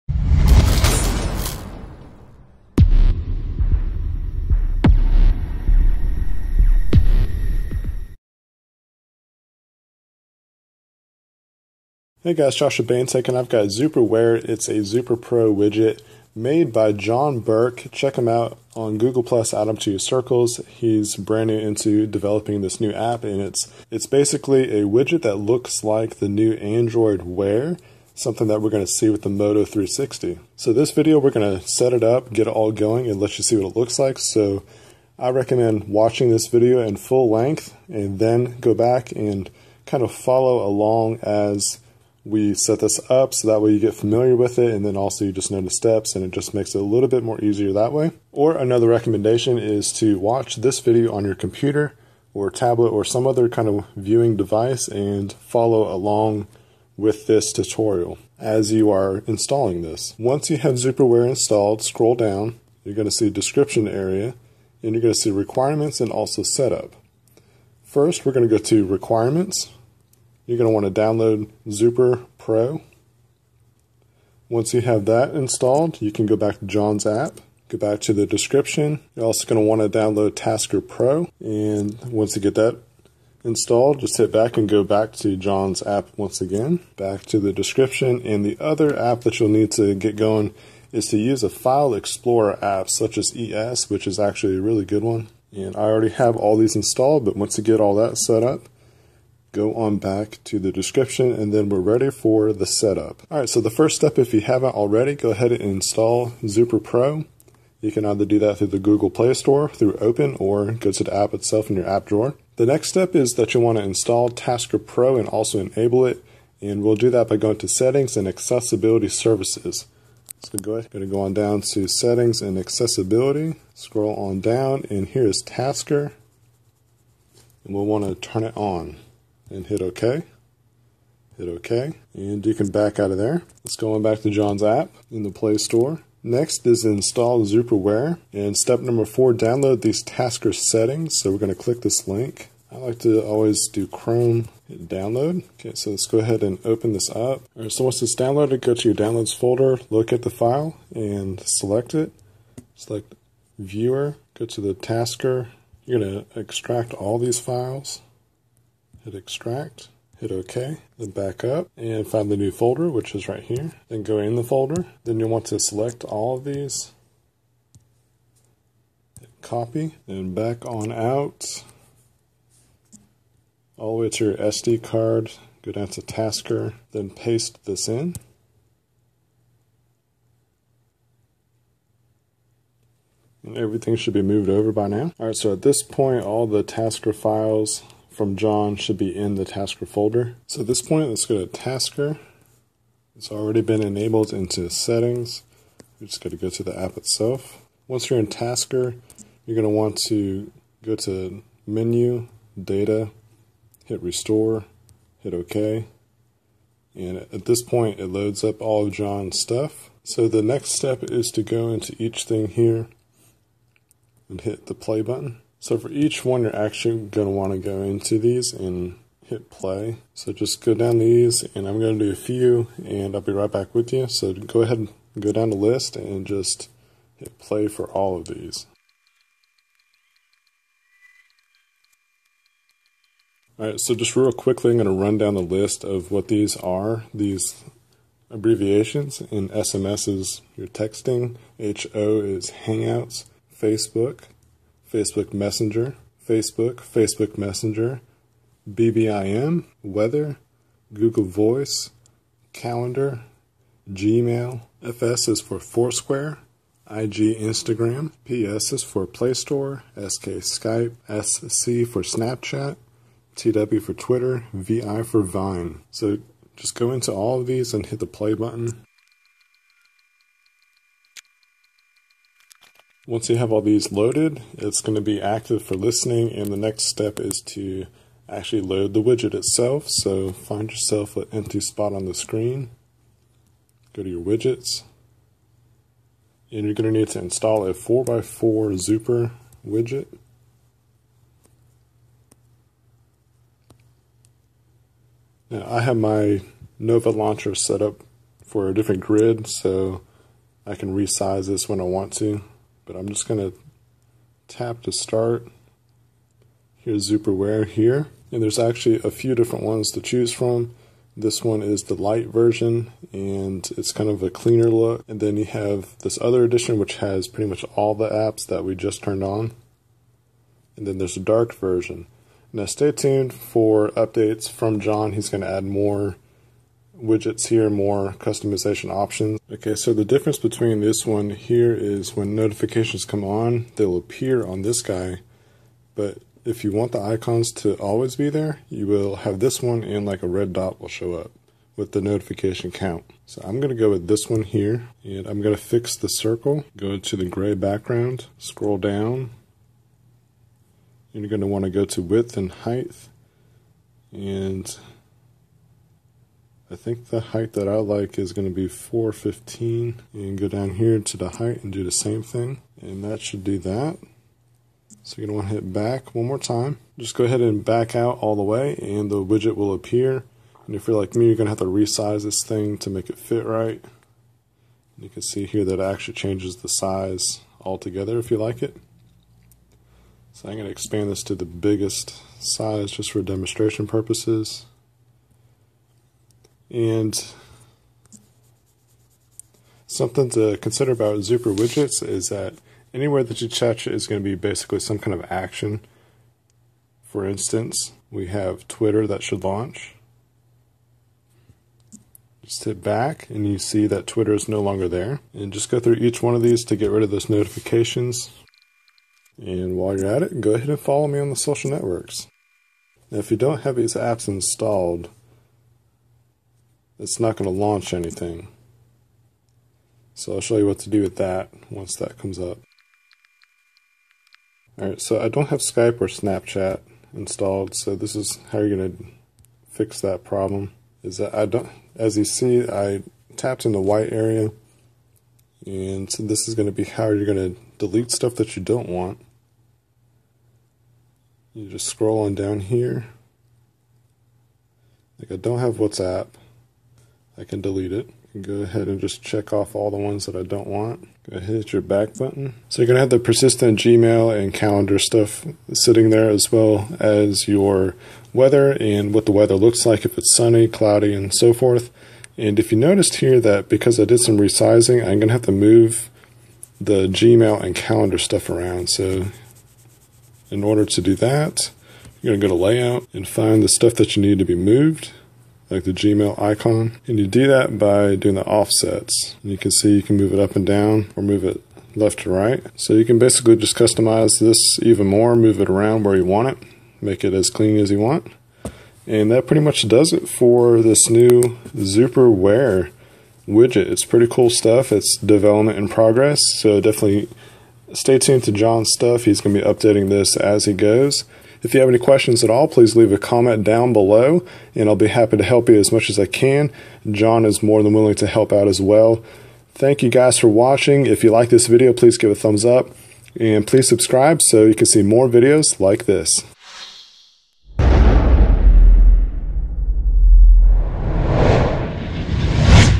Homescreen Makeover Series: Zooper Wear - Zooper Pro Widget (Android Wear Widget)
This Zooper Pro widget is made to look like the new Android Wear smartwatches.

This video will help walk you through how to install and setup the widget.

EDIT: Weather? It's WhatsApp. Also SMS is only for Sliding Messaging. Zooper doesn't require Tasker for normal/stock SMS